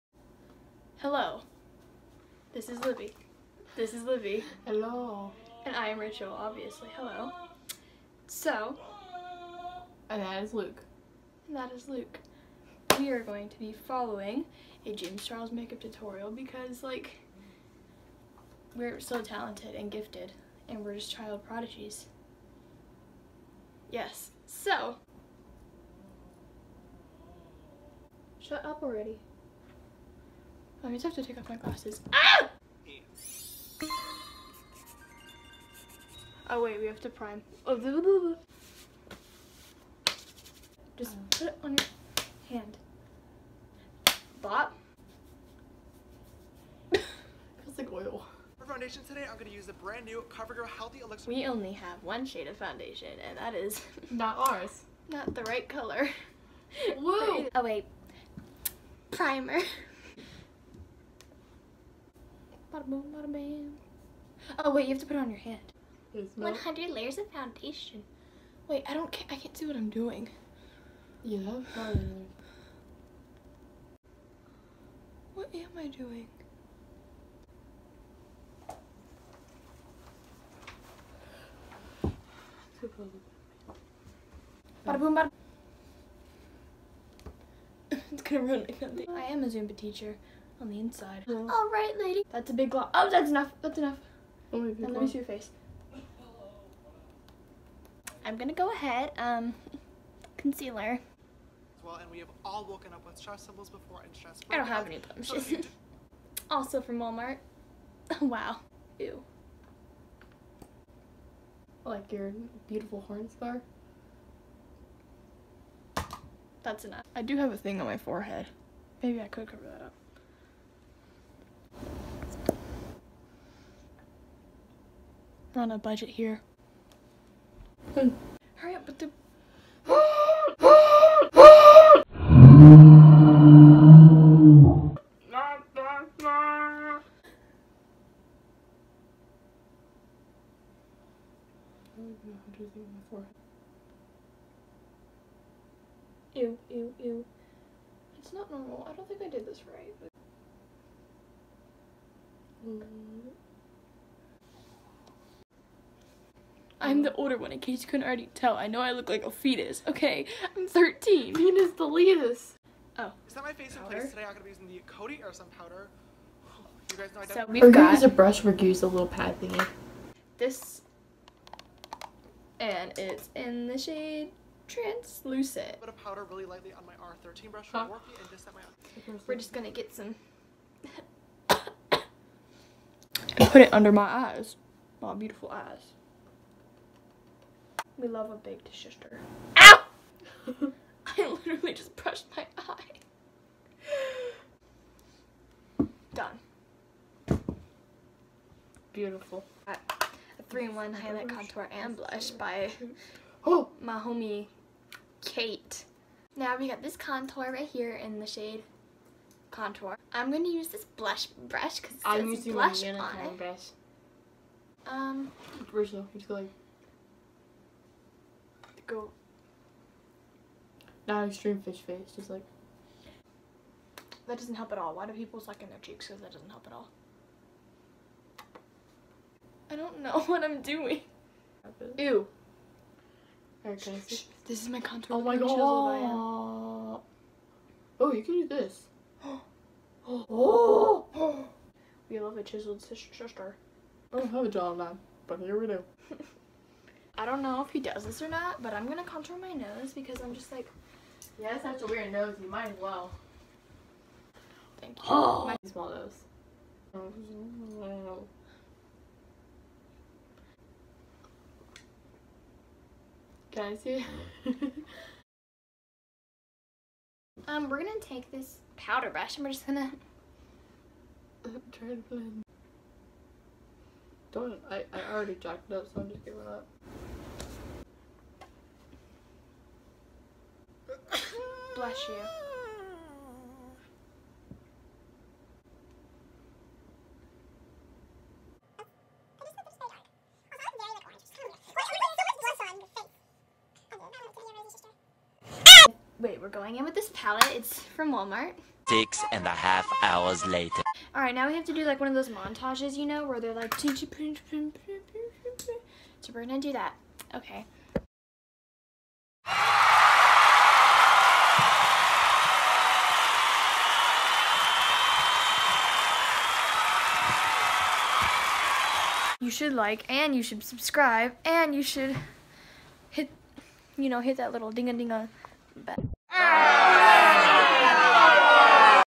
MIDDLE SCHOOLERS FOLLOW JAMES CHARLES TUTORIAL!! (KILLED IT) *NOT CLICKBAIT* WATCH UNTIL THE END!!!!
hit that little dingadinga bell

bye

shredded lettuce